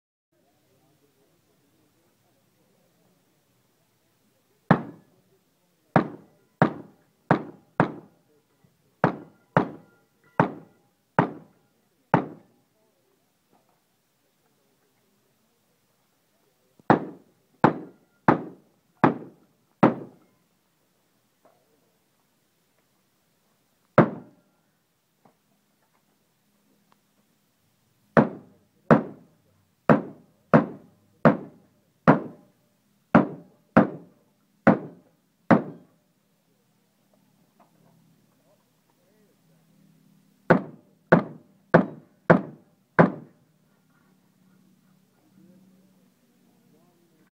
Adkal MKA1919 in action
Yancy Koester using an Adkal MKA1919 customized by Firebird Precision on an all-shotgun stage at the 2012 Superstition Mountain Mystery Three Gun match. I had a chance to run a few rounds through it and worked great. The gun felt and ran just like an AR: All the controls were right where they should be except the charging handle, and 12 gauge sure kicks more than .223!